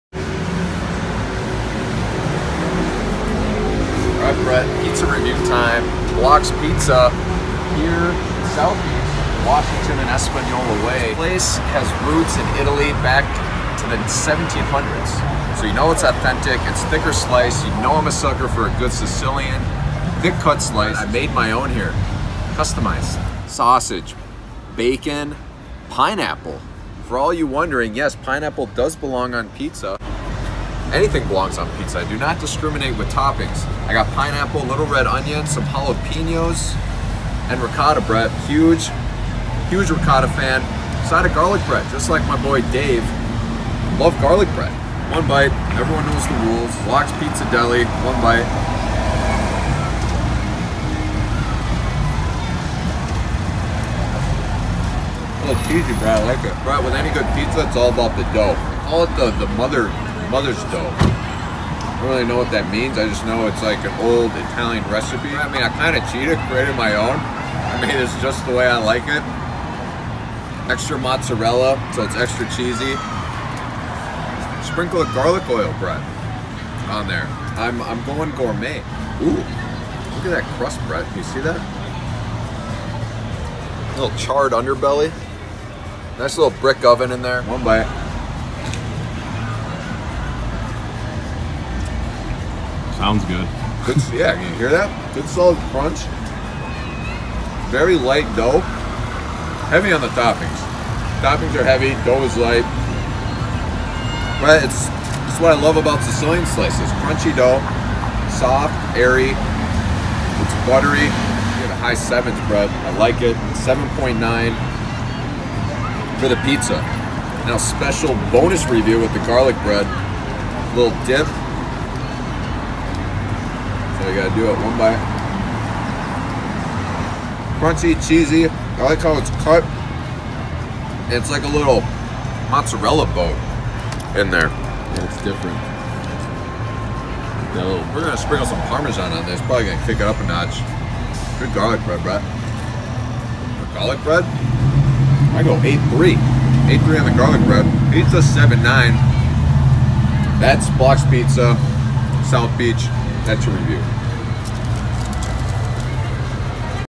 Pizza Review at Blocks Pizza Deli / Miami Beach
🍕 sausage, bacon, ricotta, pineapple, jalapeño
⭐️ 7.9 pizza
⭐️ 8.3 garlic bread
💸 $6
💭 Never knew what “Mother Dough” was but after eating this pizza, safe to say I’m now a BIG fan. Some of the lightest, fluffiest pizza dough that’s caressed my taste buds... and with the crunchy bottom👌🏼 You don’t have to know the history of the dough, all you need to know is that it’s delicious and what I believe is one of the best slices on the beach. And don’t sleep on their garlic bread! Incredible little cheesy garlic canoes. Sauced approved 🙌🏼